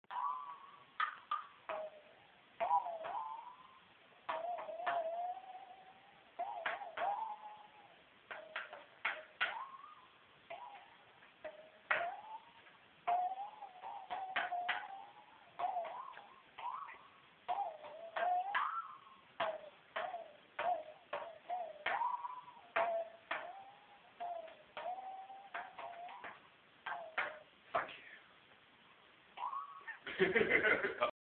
lazza playing the saw part 2
this lad continues to show how talented he is